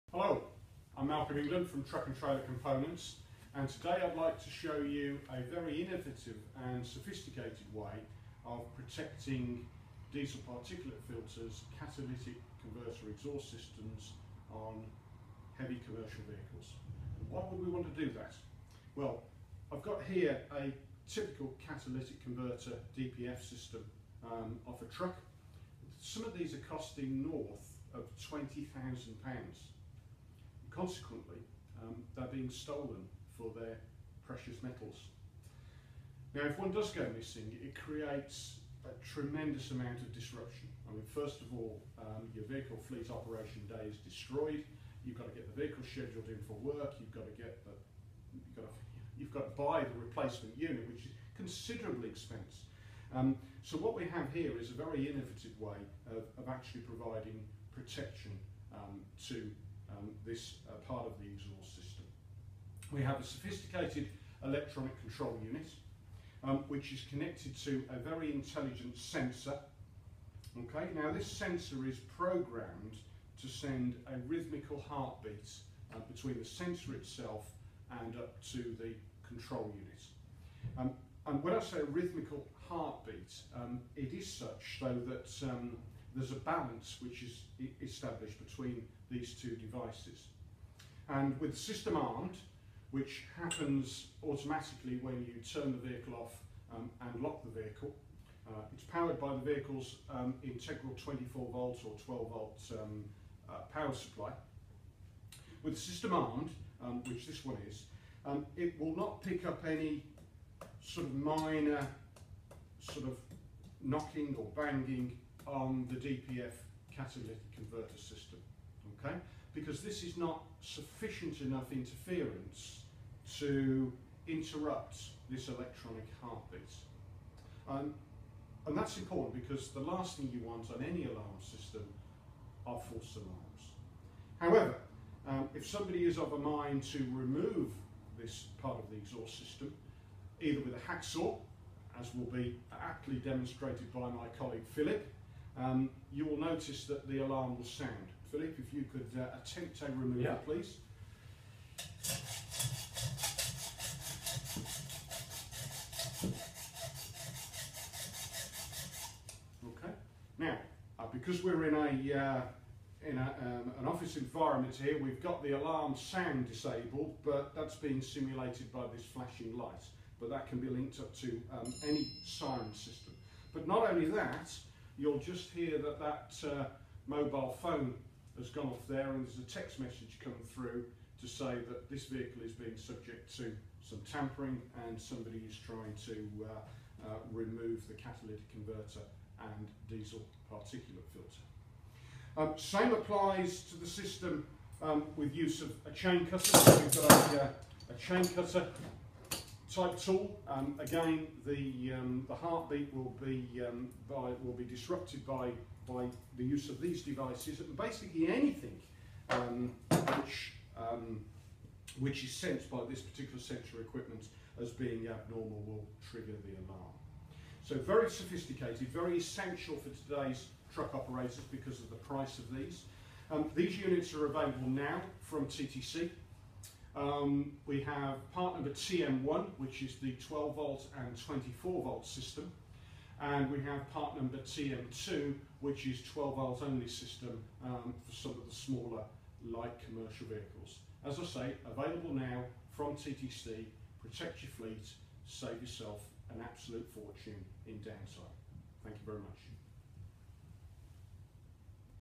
Truckminder: Catalytic converter/DPF protection
Malcolm England demonstrates how the Truckminder system protects your vehicles' catalytic converter and diesel particulate filter from being stolen.

The Truckminder system is available from TTC in two versions,
12/24v, part number TM1, and 12v only, part number TM2.

The first in a series demostrating the Truckminder alarm system.